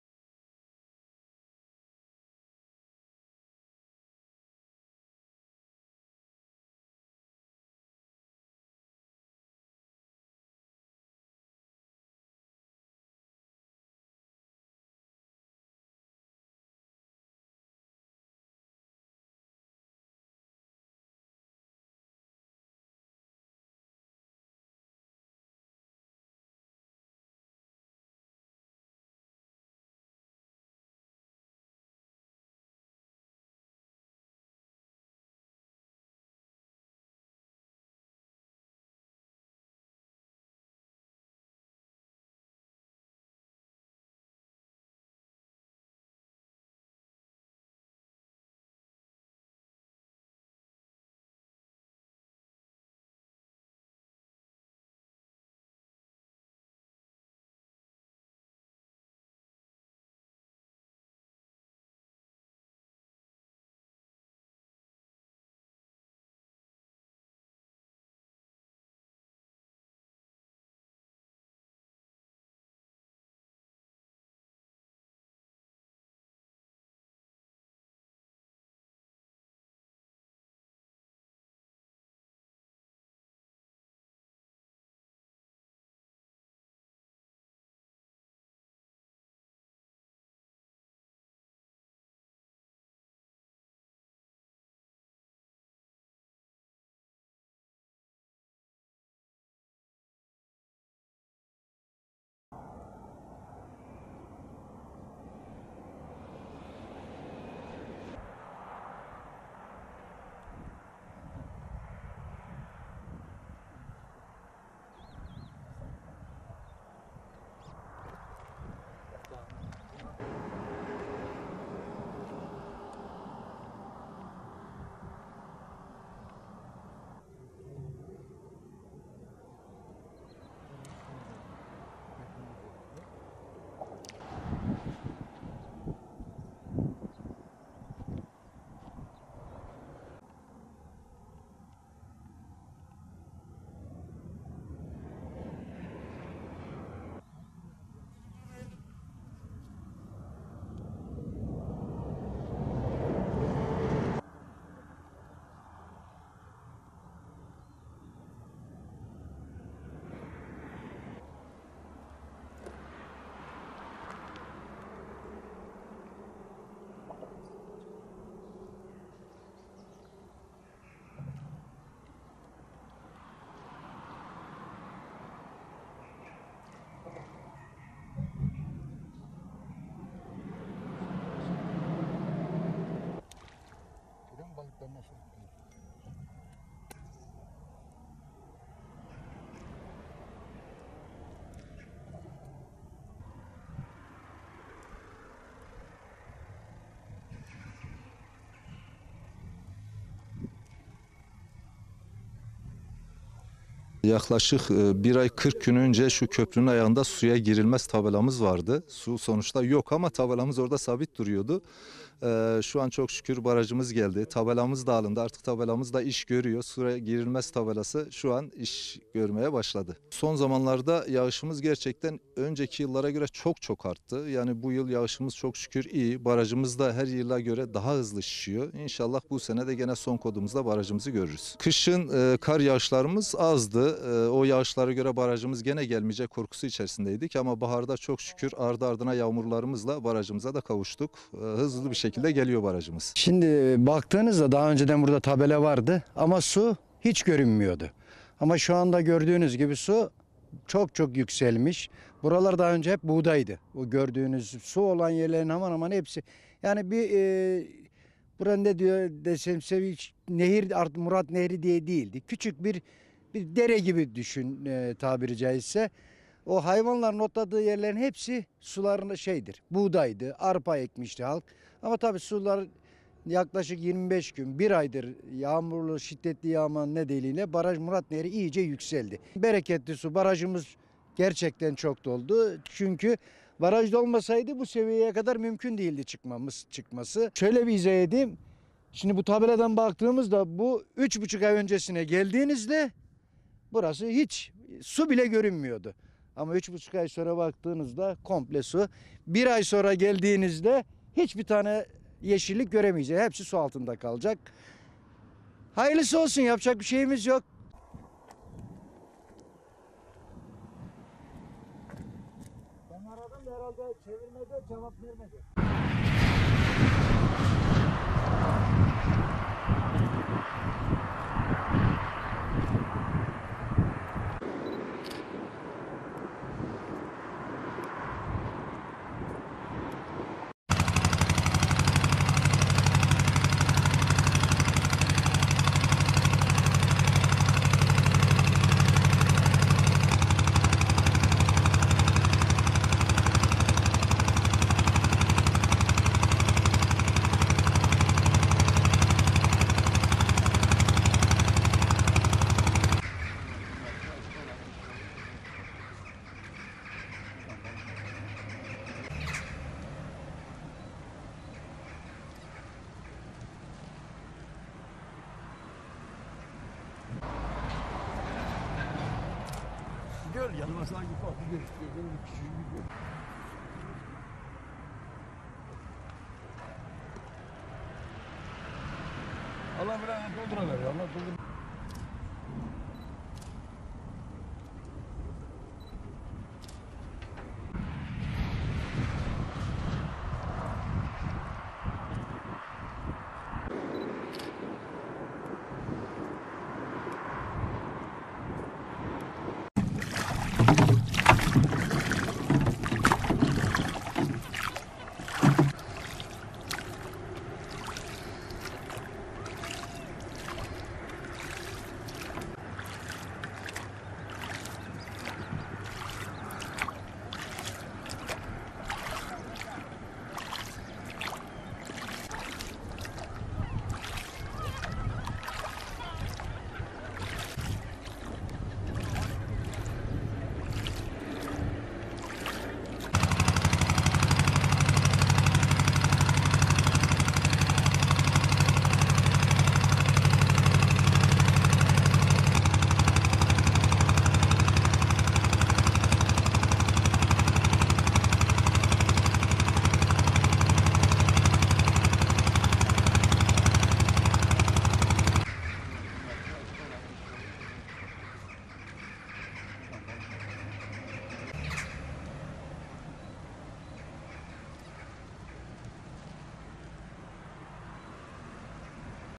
Yağışlar bereket getirdi: Artık o tabeladan su görünüyor
Elazığ’da 3,5 ay önce çekilen görüntüde "Suya girmek yasaktır" yazısı var fakat su yoktu. Son zamanlarda bölgede etkili olan yağışlarla birlikte artık o tabeladan su görünmeye başladı.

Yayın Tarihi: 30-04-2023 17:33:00